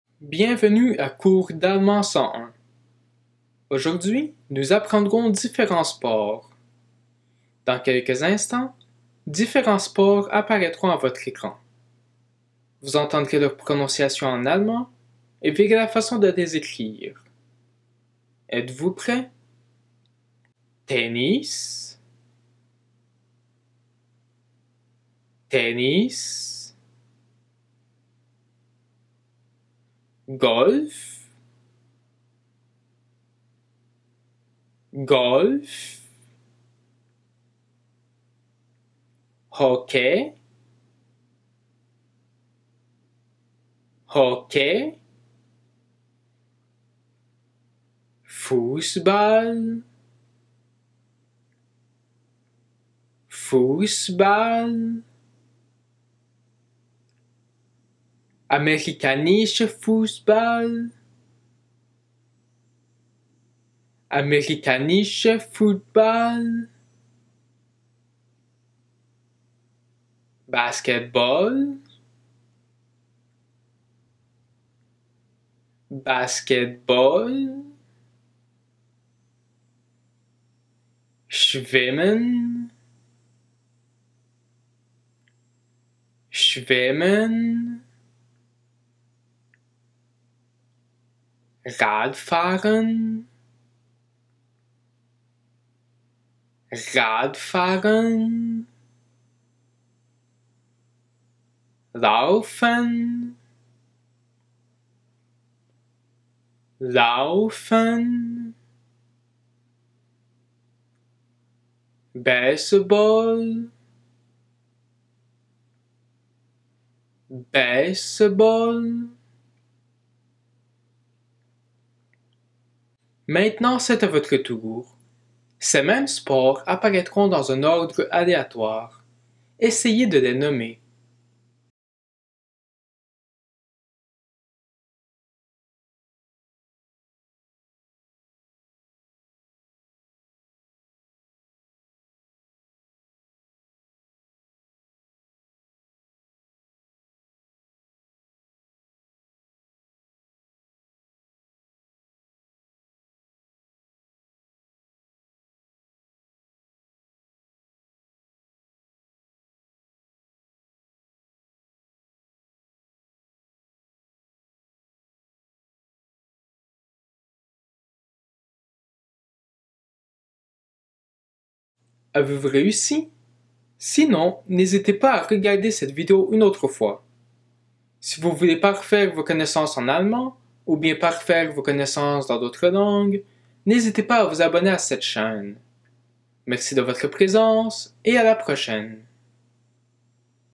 Cours d'Allemand 101: Les Sports
Apprenez le nom des sports en Allemand